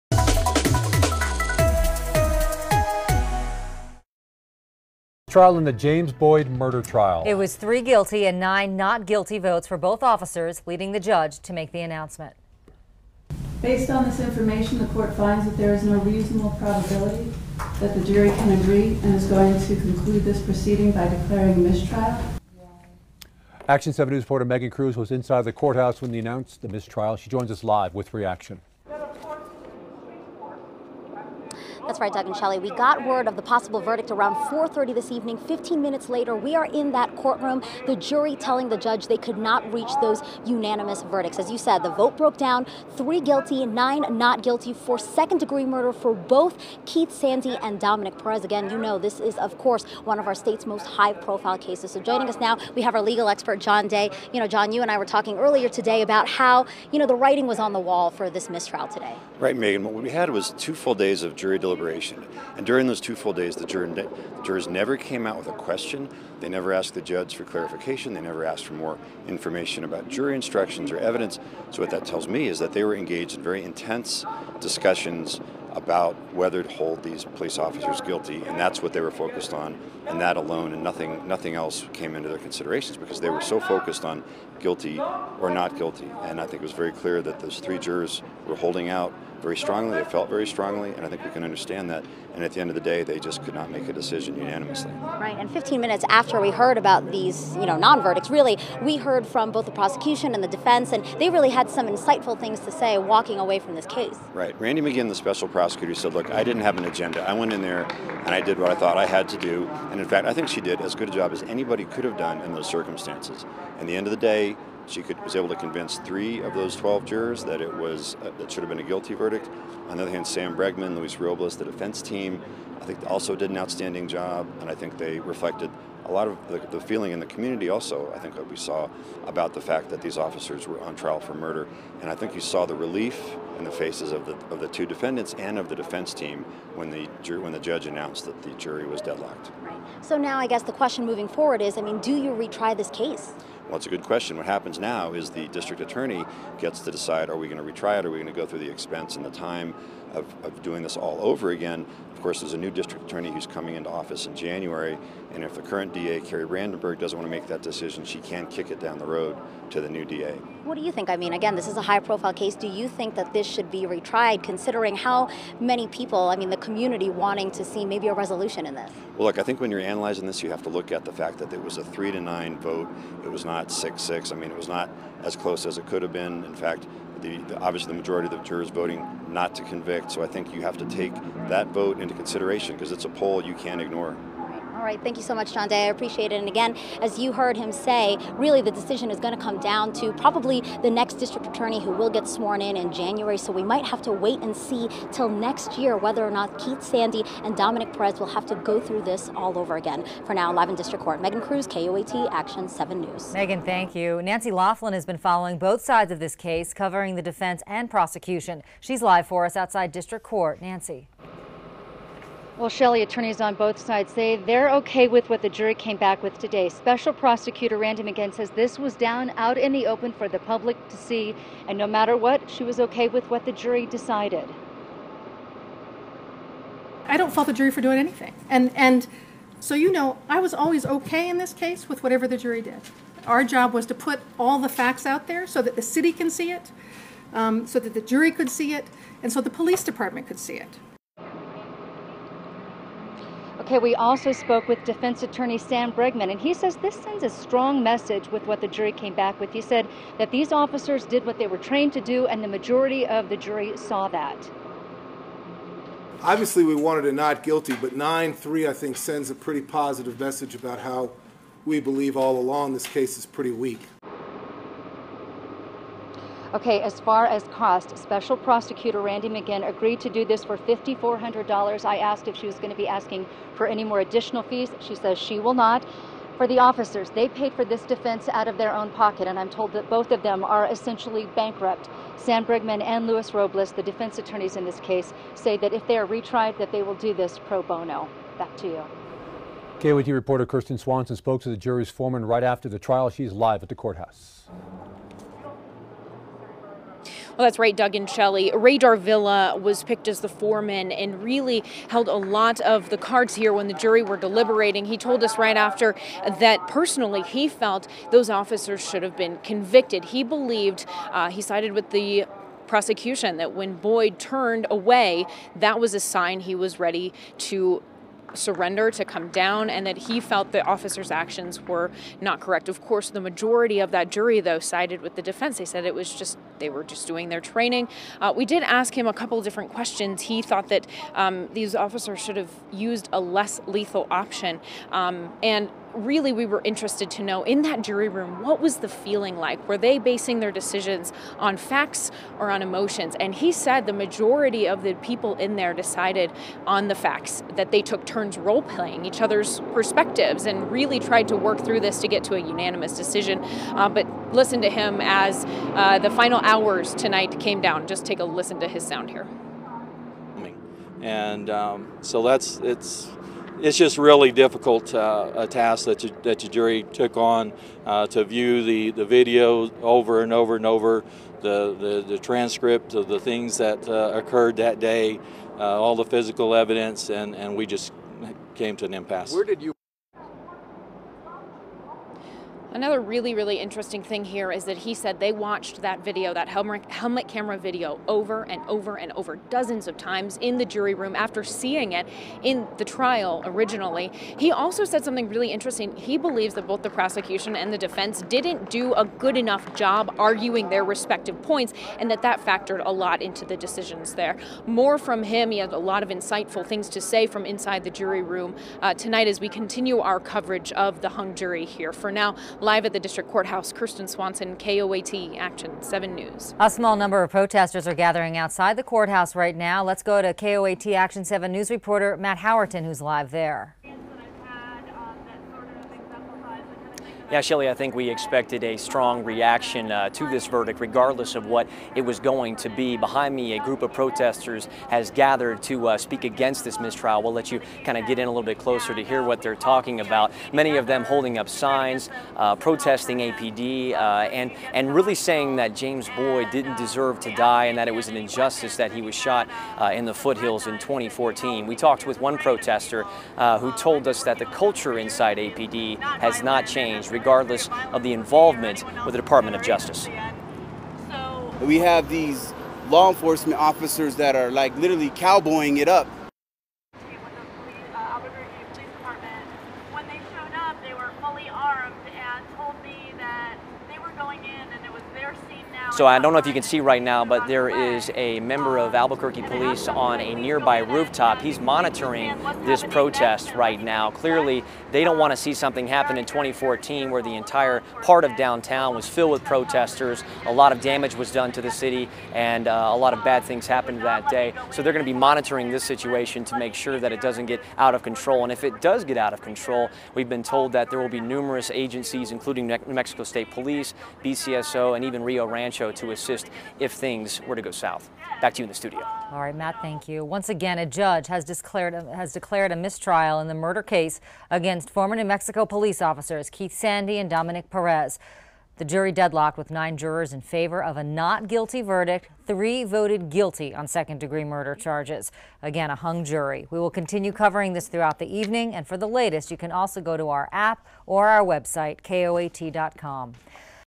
Team Coverage: James Boyd mistrial
Judge Alisa Hadfield made the declaration of a mistrial on Tuesday after three days of deliberations in the case involving the 2014 on-duty killing of a mentally ill homeless man in Albuquerque.